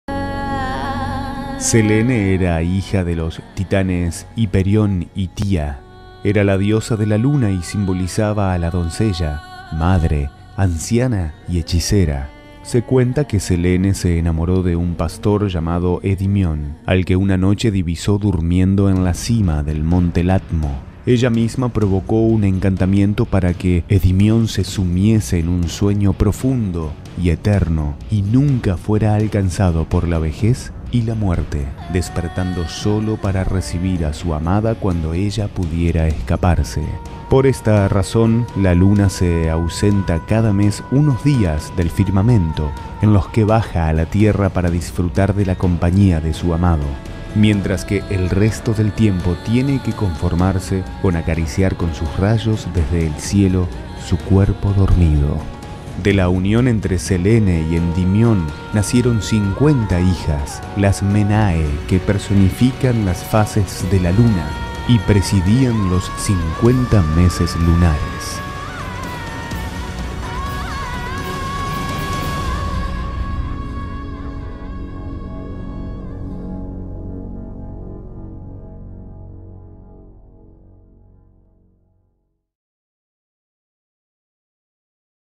MISS VILLA ELISA 2016- Diosa Selene/ Mitología Griega- Postulante Nº12 Idalis Deymonaz
Filmación y Edición: Ignacio Barragán_ "Estudio Creativa"--Idea. Coordinación, Textos, Utileria: Rosana Villalonga- Vestimenta: Diseños Flavia Villon- Maquillaje: virginia Rocha Make Up- Voz: Mariano Bravo- Extras: Julián Noir.